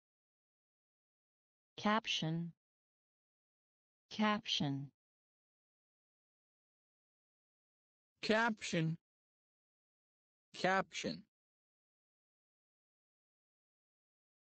How to Pronounce CAPTION in American English
Learn how to pronounce caption in American English. Learn how to say caption and its meaning. All Pronunciations videos are in multiple voices - male and female.

Practice the Pronunciation of caption. English Vocabulary and Pronounciation for TOEFL GMAT GRE IELTS.

#:caption:#